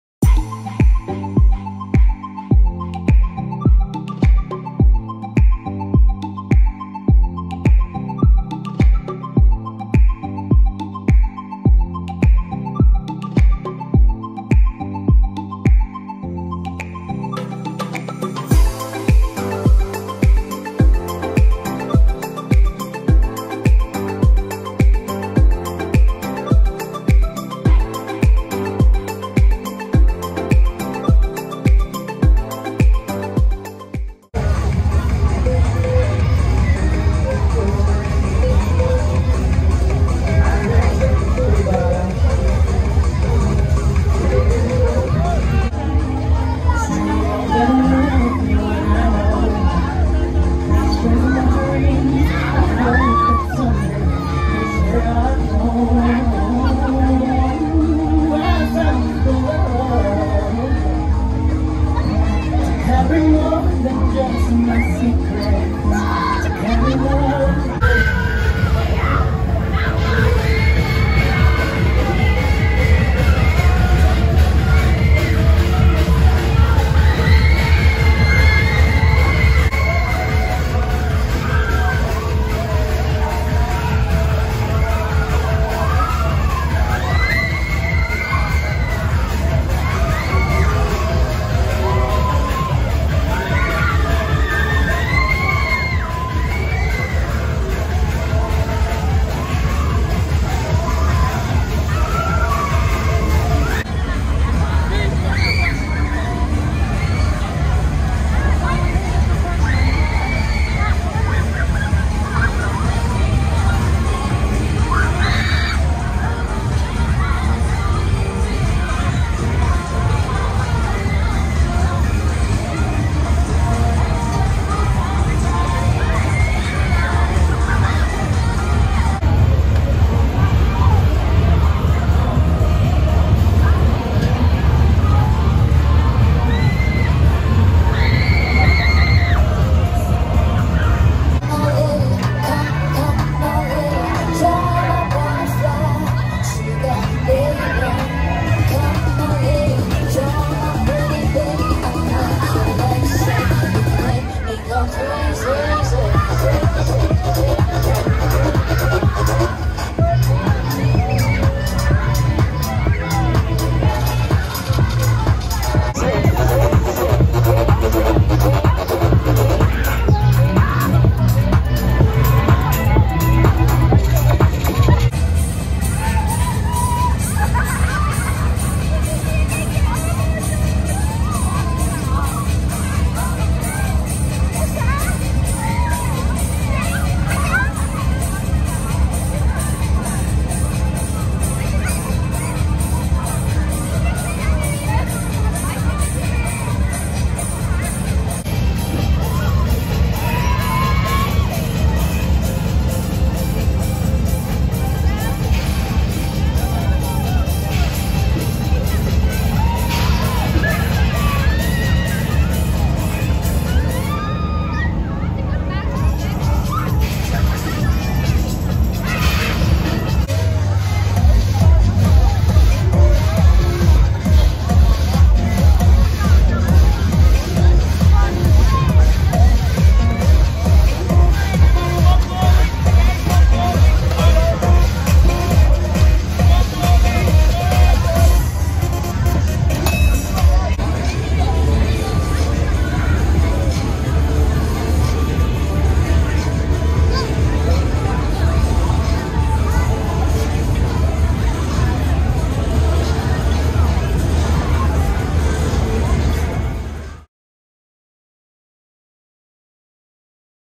Penistone Funfair July 2021
This Evening We Visited Penistone Funfair Which Was An Amazing Fair, Brilliant Showmen, Imaculate, Stunning Rides Too! Make Sure To Get Down There And Support Them 🕺